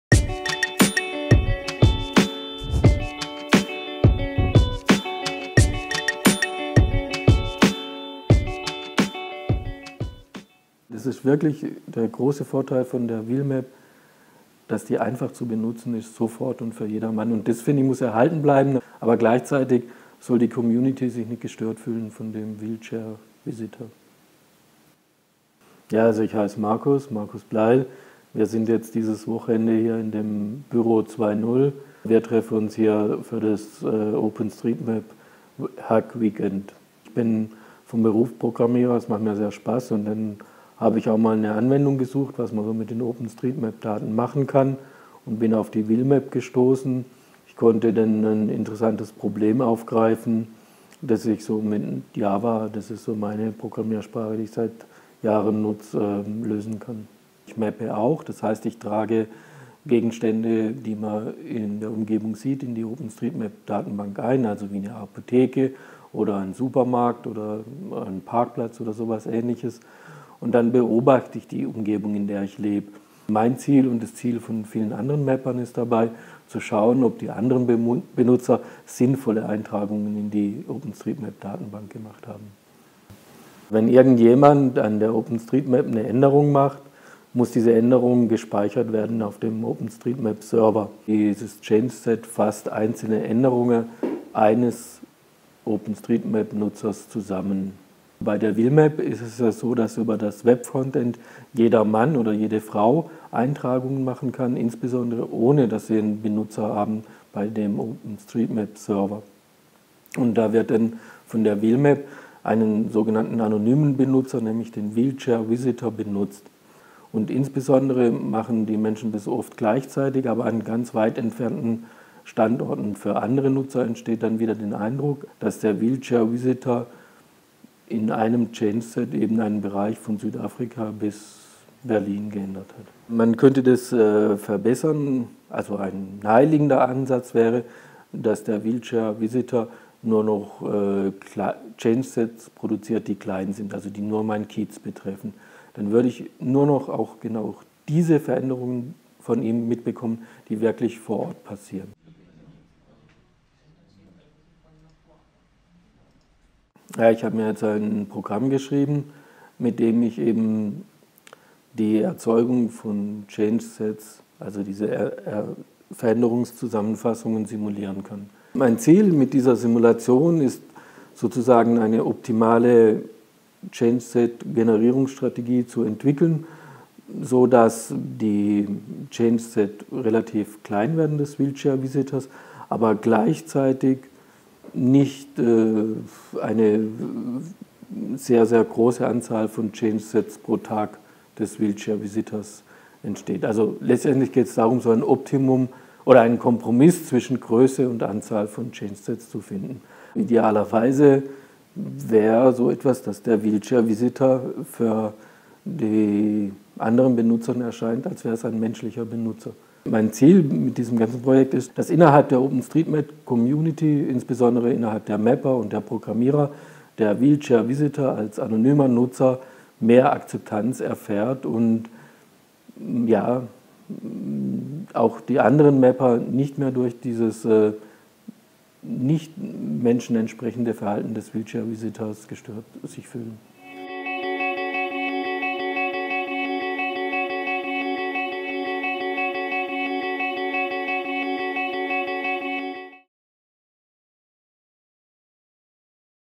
Vom Umgang mit dem wheelchair_visitor -- Wheelmap.org und die OpenStreetMap-Community
Interview mit einem Teilnehmer des OpenStreetMao Hack Weekend Berlin 26. Januar 2014. Thema ist OpenStreetMap und wie mit dem Pseudo-User wheelchair_visitor umgegangen werden kann so dass die OSM-Nutzer nicht gestört werden vom Projekt Wheelmap.